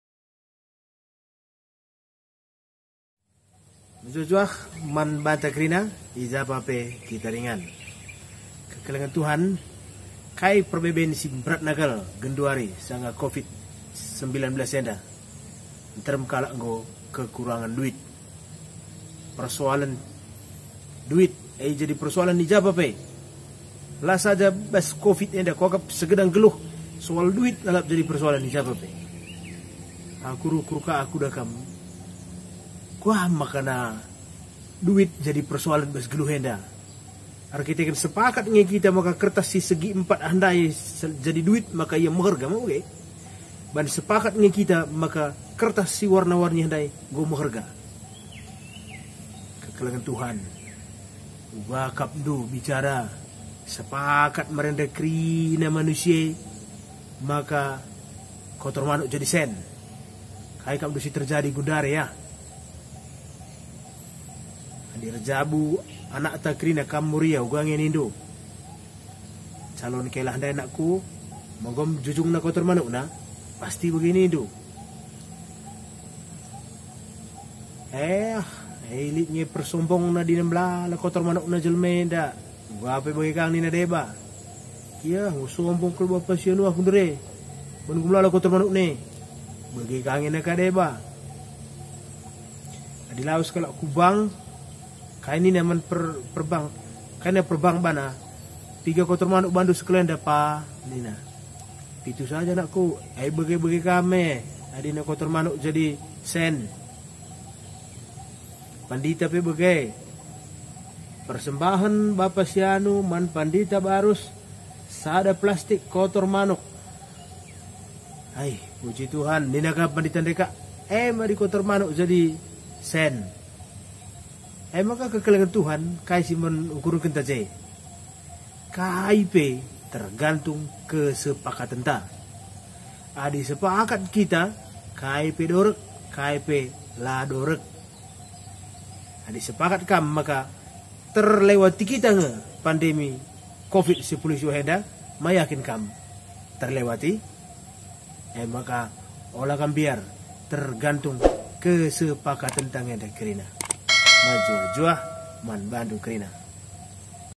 Renungen Jenaka Karo | Bicara Kotor Manuk Jadi Duit
Renungan Jenaka Karo "Bicara Kotor Manuk Jadi Duit" (Seandainya Kotoran Ayam Jadi Uang) mengandung pesan bahwa banyak hal di dunia ini menjadi pentinh karena disepakati bersama oleh masyarakat, selembar kertas warna-warni bergambar orang menjadi berharga karena ia disepakati sebagai alat tukar, seandainya kotoran ayam disepakati pula sebagai uang...maka benda kotor itu seketika akan menjadi berharga dan menjadi ukuran kesuksesan seseorang